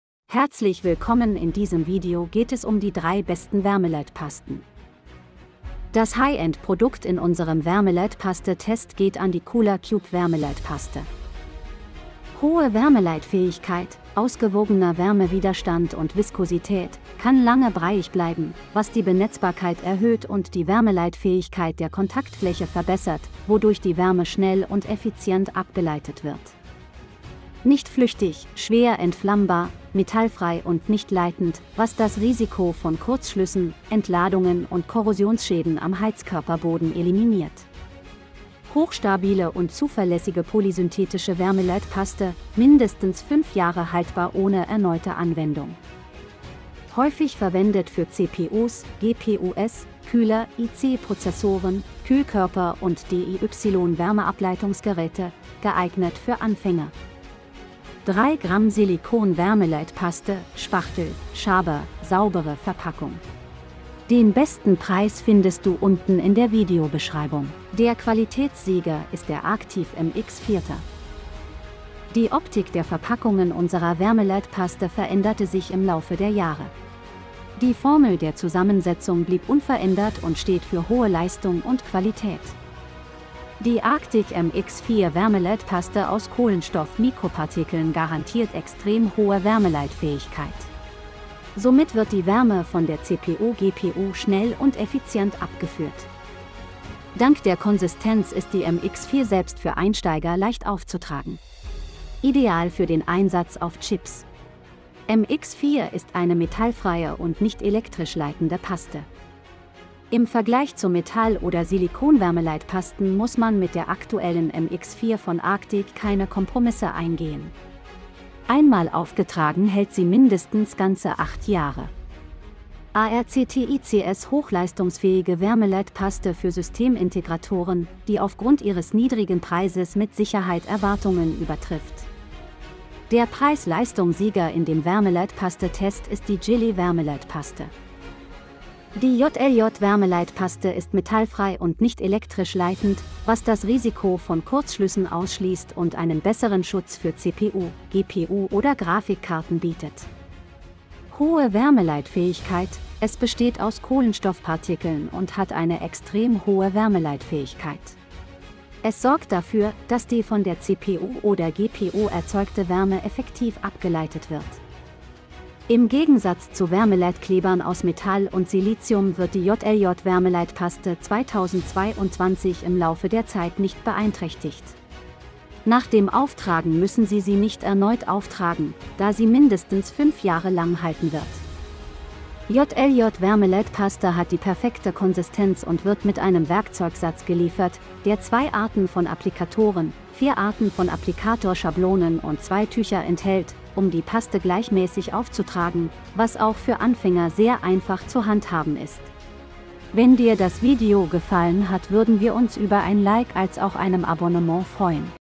» Wärmeleitpaste Test ∥ Die besten 3 Wärmeleitpasten 2023
Das High End Produkt in unserem Wärmeleitpaste Test geht an die Cooler Cube Wärmeleitpaste.

Hohe Wärmeleitfähigkeit, ausgewogener Wärmewiderstand und Viskosität, kann lange breiig bleiben, was die Benetzbarkeit erhöht und die Wärmeleitfähigkeit der Kontaktfläche verbessert, wodurch die Wärme schnell und effizient abgeleitet wird.
Nicht flüchtig, schwer entflammbar, metallfrei und nicht leitend, was das Risiko von Kurzschlüssen, Entladungen und Korrosionsschäden am Heizkörperboden eliminiert.
Hochstabile und zuverlässige polysynthetische Wärmeleitpaste, mindestens 5 Jahre haltbar ohne erneute Anwendung.
Häufig verwendet für CPUs, GPUs, Kühler, IC Prozessoren, Kühlkörper und DIY Wärmeableitungsgeräte, geeignet für Anfänger.
3 Gramm Silikon Wärmeleitpaste, Spachtel, Schaber, saubere Verpackung.

Der Qualitätssieger ist der Arctiv MX 4.

Die Optik der Verpackungen unserer Wärmeleitpaste veränderte sich im Laufe der Jahre. Die Formel der Zusammensetzung blieb unverändert und steht für hohe Leistung und Qualität.
Die ARCTIC MX 4 Wärmeleitpaste aus Kohlenstoff Mikropartikeln garantiert extrem hohe Wärmeleitfähigkeit. Somit wird die Wärme von der CPU GPU schnell und effizient abgeführt.
Dank der Konsistenz ist die MX 4 selbst für Einsteiger leicht aufzutragen. Ideal für den Einsatz auf Chips. MX 4 ist eine metallfreie und nicht elektrisch leitende Paste.
Im Vergleich zu Metall  oder Silikonwärmeleitpasten muss man mit der aktuellen MX 4 von ARCTIC keine Kompromisse eingehen. Einmal aufgetragen hält sie mindestens ganze 8 Jahre.
ARCTICs hochleistungsfähige Wärmeleitpaste für Systemintegratoren, die aufgrund ihres niedrigen Preises mit Sicherheit Erwartungen übertrifft.
Der Preis Leistung Sieger in dem Wärmeleitpaste Test ist die Jily Wärmeleitpaste.

Die JLJ-Wärmeleitpaste ist metallfrei und nicht elektrisch leitend, was das Risiko von Kurzschlüssen ausschließt und einen besseren Schutz für CPU, GPU oder Grafikkarten bietet.
Hohe Wärmeleitfähigkeit, es besteht aus Kohlenstoffpartikeln und hat eine extrem hohe Wärmeleitfähigkeit. Es sorgt dafür, dass die von der CPU oder GPU erzeugte Wärme effektiv abgeleitet wird.
Im Gegensatz zu Wärmeleitklebern aus Metall und Silizium wird die JLJ-Wärmeleitpaste 2022 im Laufe der Zeit nicht beeinträchtigt. Nach dem Auftragen müssen Sie sie nicht erneut auftragen, da sie mindestens 5 Jahre lang halten wird.
JLJ Wärmeleitpaste hat die perfekte Konsistenz und wird mit einem Werkzeugsatz geliefert, der 2 Arten von Applikatoren, 4 Arten von Applikatorschablonen und 2 Tücher enthält, um die Paste gleichmäßig aufzutragen, was auch für Anfänger sehr einfach zu handhaben ist.